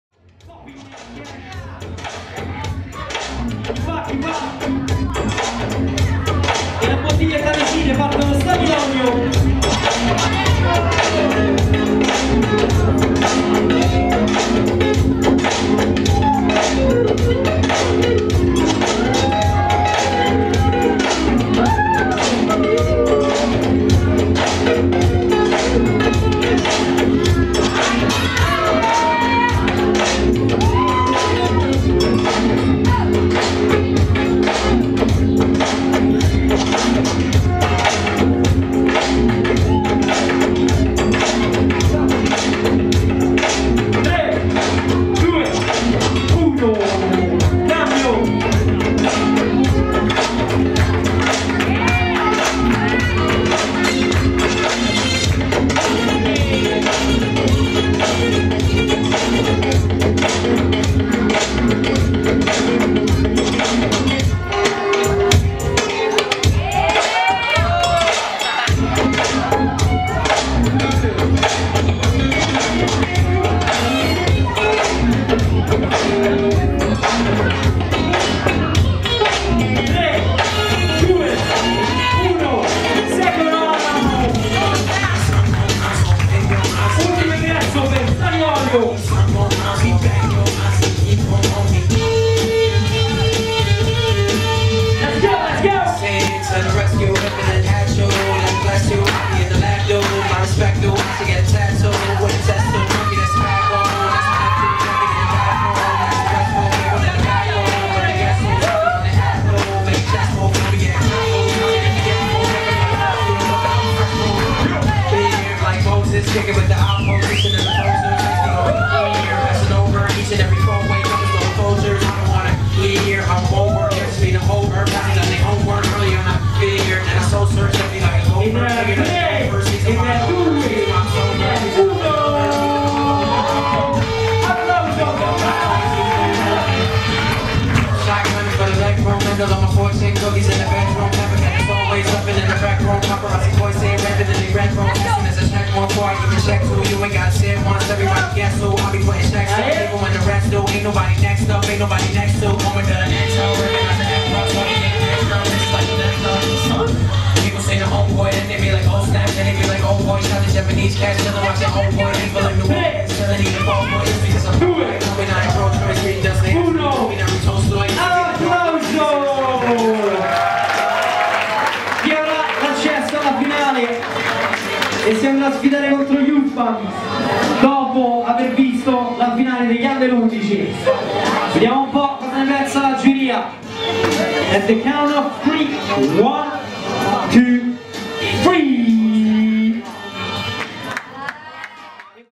Protocollo VS Stanlio&Ollio SEMIFINAL UNDER16
Instagram: @a_ day_ with_ the_ street

Host - Piesse LaTrucha
DJ - Mr Meth KLANDESKILLZ CREW

~POPPING 1vs1 Open
Judges: Neji / Michel

~HIP HOP 1vs1 Open
Judges: Shot/ Steve/ Wally

~MIXSTYLE 2vs2 under 11 & under 16
Judges: Sandra Col / LaTrucha